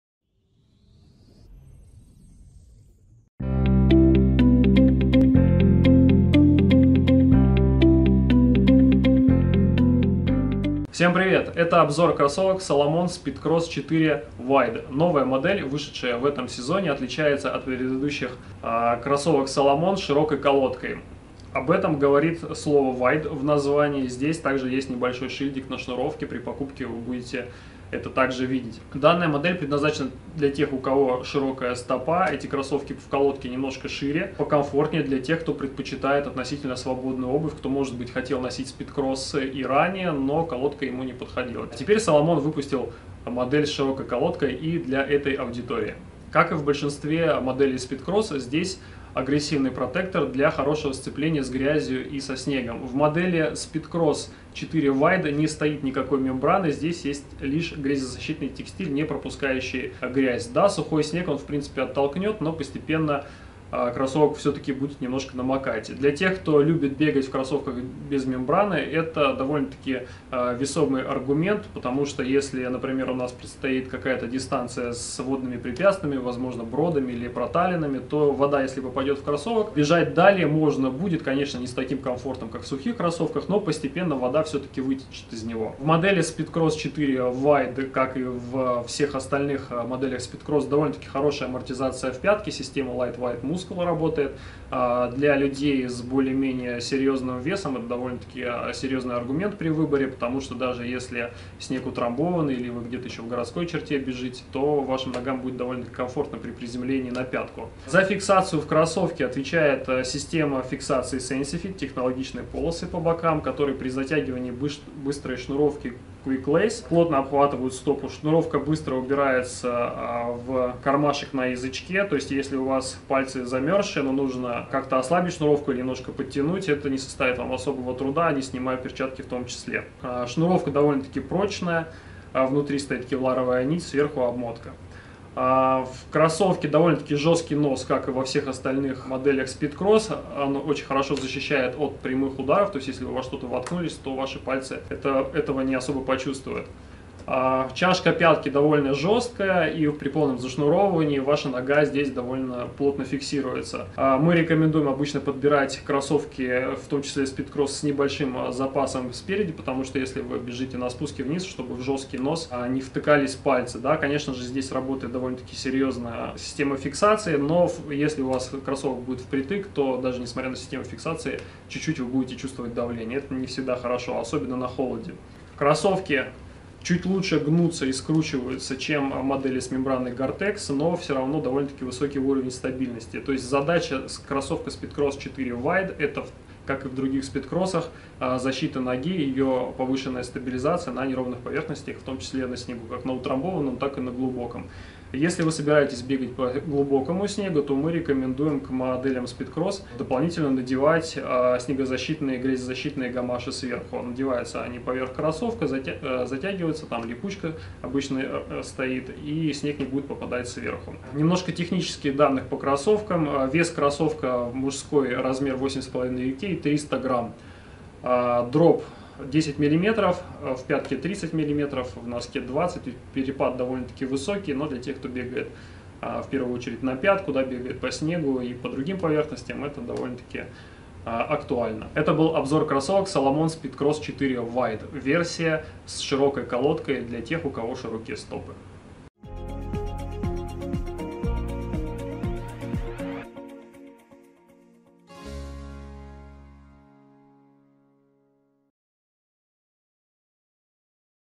Кроссовки Salomon Speedcross 4 Wide. Обзор
Модель с широкой колодкой для тренировок и соревнований по пересечённой местности с максимальным сцеплением со сложными поверхностями и хорошей амортизацией.

• рекомендации по условиям использования: грязные тропы, рыхлые поверхности, снег, мягкие лесные и горные грунты. Благодаря особенностям строения Salomon Speedcross 4 подходит в первую очередь для тренировок, для хайкинга - быстрых походов в стиле Fast&Light. Их работа на соревнованиях будет зависеть от вашего опыта и скорости;

• не рекомендуем: модель не подходит для бега по льду, повседневной носки по твёрдым поверхностям. Плохо подходит для бега по асфальту и другим твёрдым поверхностям по причине более быстрого износа протектора;

• область применения: скайраннинг, ориентирование, рогейны, хайкинг, лесные и горные маршруты с грязными, не стабильными поверхностями, забеги на различные дистанции, в том числе марафоны и ультра-трейлы по пересеченной местности;

• рекомендуемый уровень подготовки: любой уровень подготовки.

Статья по теме "Как выбрать кроссовки для бега" в нашем блоге